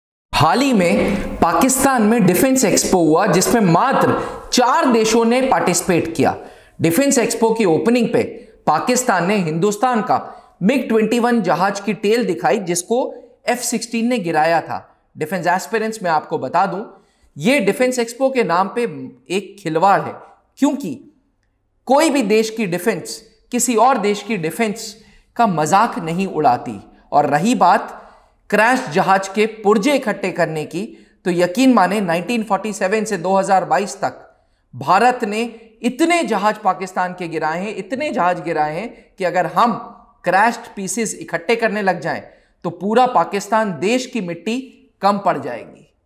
Pakistan Displays India’s MiG-21 Bison’s Tail Shot Down By PAF F-16 Fighter Jet At Its Defense Expo
Abhinandan’s MiG-21 attracts attention at Karachi’s defence exhibition